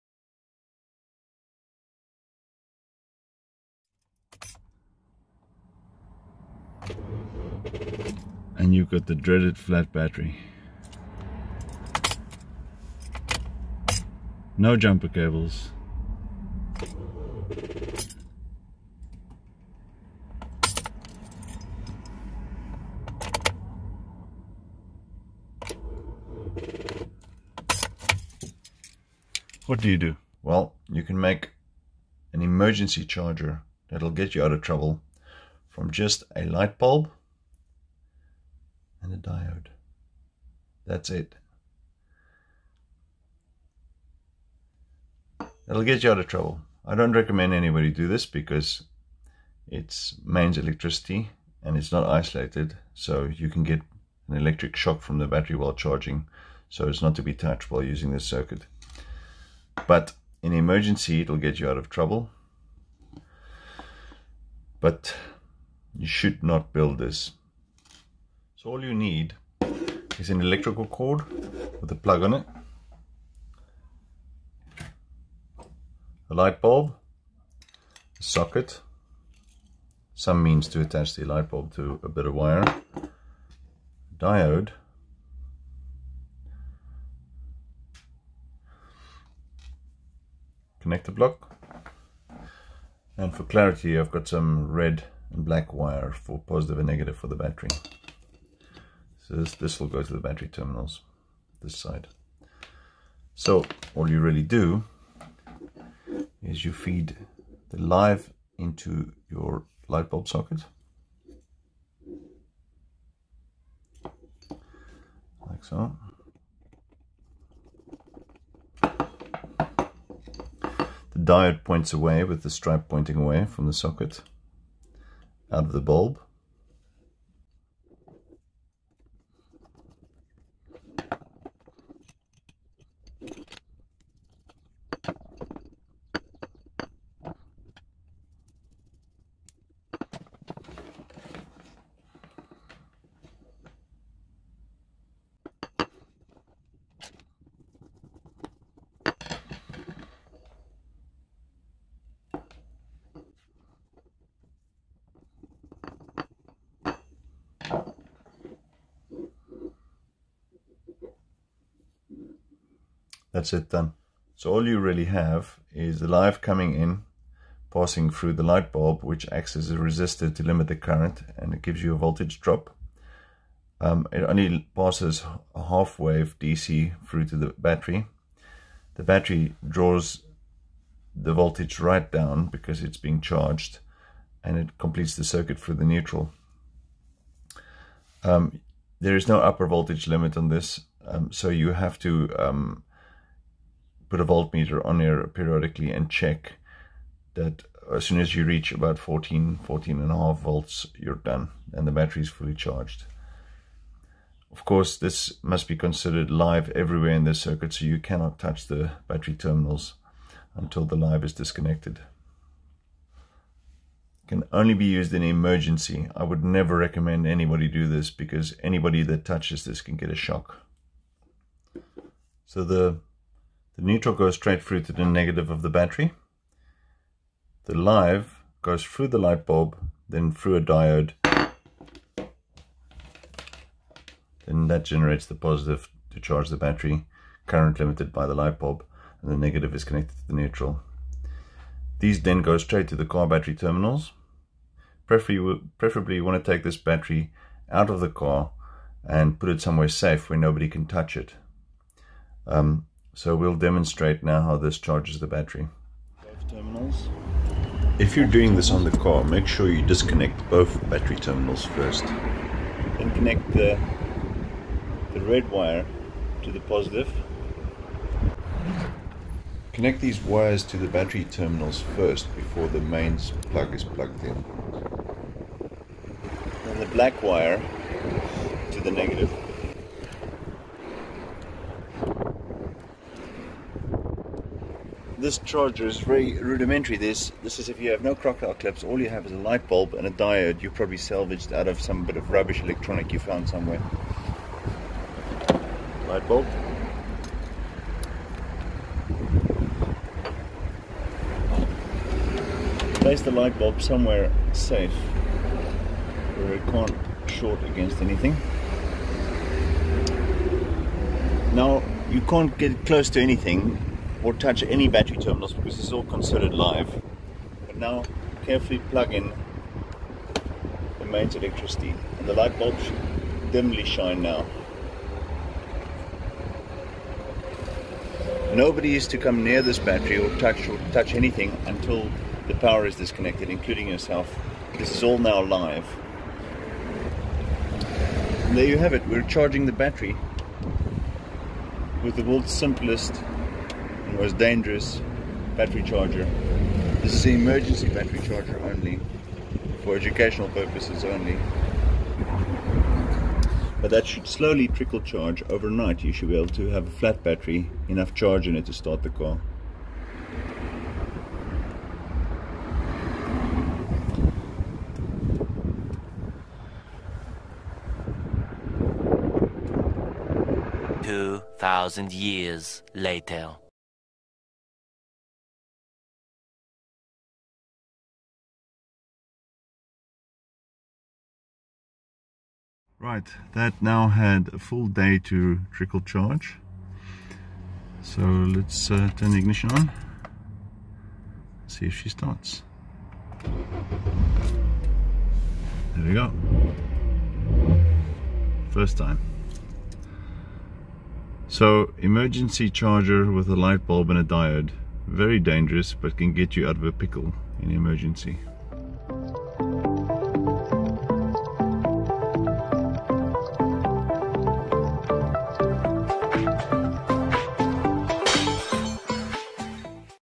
Dangerous emergency battery charger ( Don't do this!)
This is a very dangerous way to recharge your flat car battery. It's a very third world way of doing things, where health and safety goes out the window!  luckily I did it so that you don't have to. This method simply requires a old-fashioned incandescent light bulb and a diode, that's it. That's all you need to recharge your dead car battery. The circuit connects your battery directly to the mains voltage, so your battery effectively becomes live. So there is a definite electric shock risk.